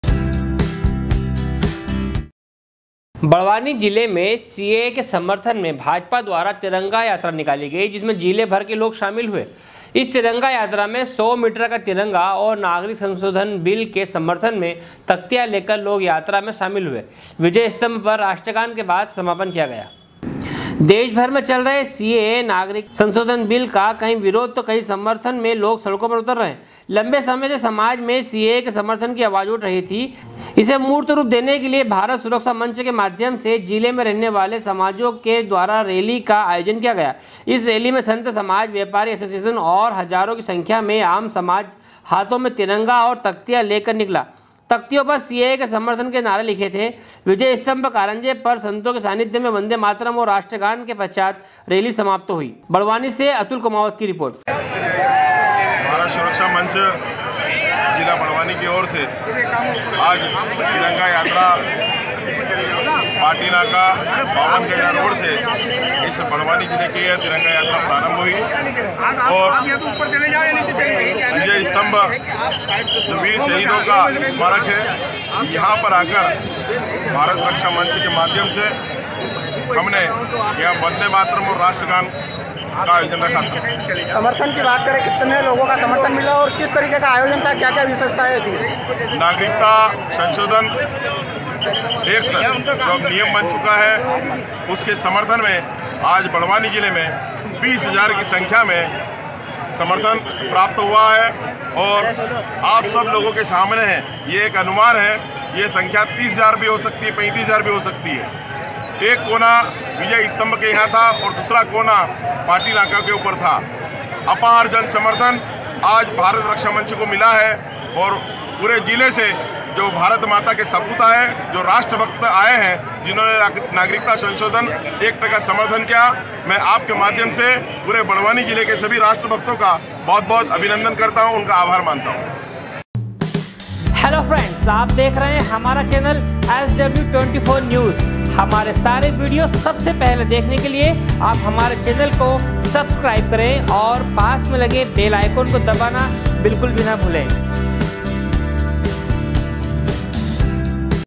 sw24news barwani, 11.01.2020
सीएए के समर्थन में भाजपा द्वारा तिरंगा यात्रा निकाली गई। जिसमें जिले भर से लोग शामिल हुए इस तिरंगा यात्रा में 100 मीटर का तिरंगा और नागरिक शंशोधन बिल के समर्थन मे तख्तियां लेकर लोग यात्रा में शामिल हुए बड़वानी से अतुल कुमावत की रिपोर्ट#sw24news